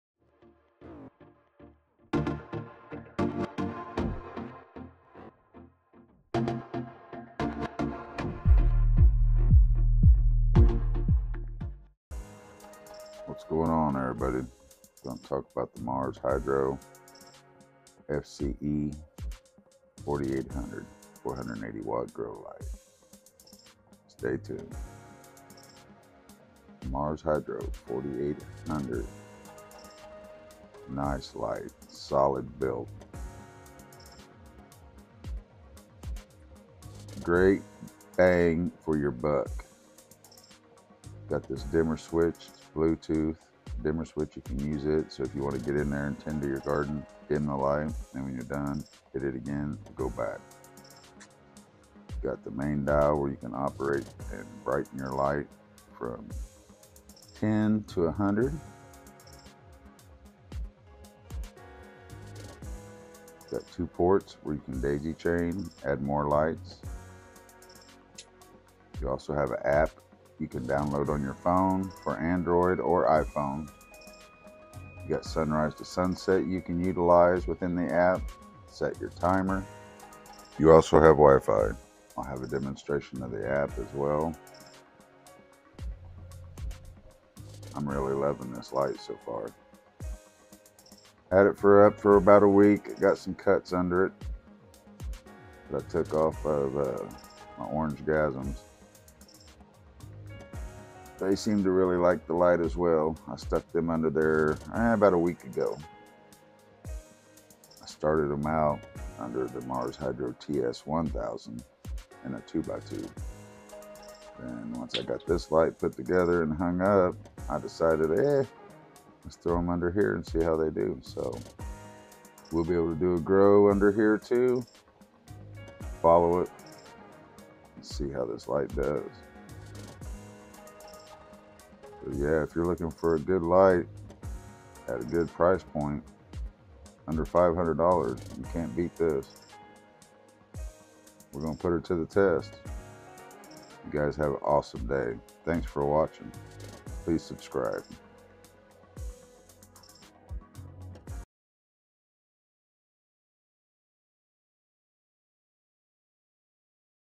Mars HydroFC-E4800 Smart Led Grow Light System
MARS HYDRO FC-E4800 BRIDGELUX 480W SMART GROW SYSTEM LED GROW LIGHT

This light is part of the FC series
FC- E4800

Personal Cultivation Coverage - 4x4 ft
Commercial Cultivation Coverage - 3x3 ft

voltage - 100-277 Vac
Frequency - 50/60HZ
PPE. 2.8  umols/J
PPF. 1341 umols/m2s

Mars Hydro
FC-E4800 Smart Led Grow Light